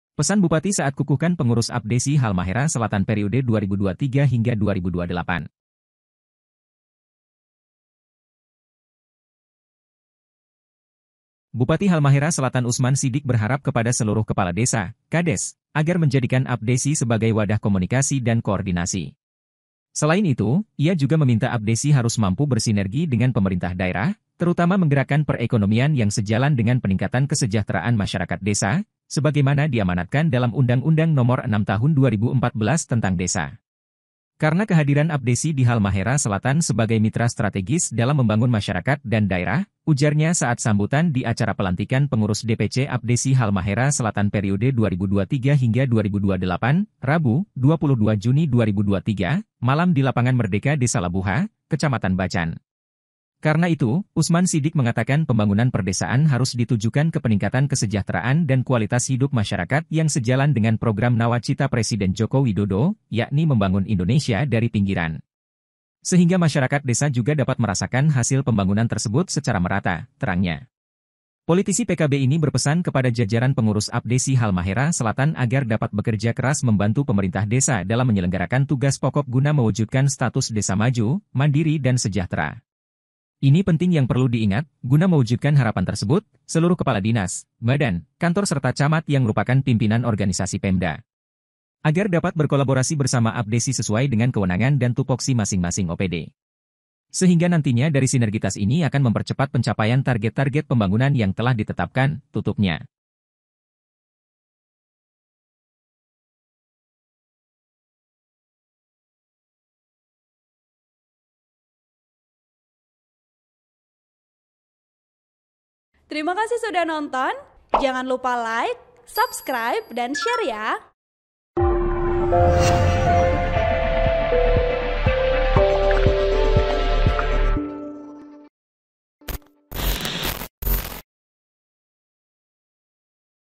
Pesan Bupati saat Kukuhkan Pengurus APDESI Halmahera Selatan Periode 2023-2028
TRIBUN-VIDEO.COM, HALMAHERA SELATAN - Bupati Halmahera Selatan Usman Sidik berharap kepada seluruh Kepala Desa (Kades) agar menjadikan APDESI sebagai wadah komunikasi dan koordinasi.
Selain itu, ia juga meminta APDESI harus mampu bersinergi dengan pemerintah daerah, terutama menggerakkan perekonomian yang sejalan dengan peningkatan kesejahteraan masyarakat desa, sebagaimana diamanatkan dalam Undang-Undang nomor 6 tahun 2014 tentang desa.

Artikel ini telah tayang di TribunTernate.com dengan judul Pesan Bupati saat Kukuhkan Pengurus APDESI Halmahera Selatan Periode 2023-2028,

TribunTernate.com
Reporter : Anto Gani
VP : shendy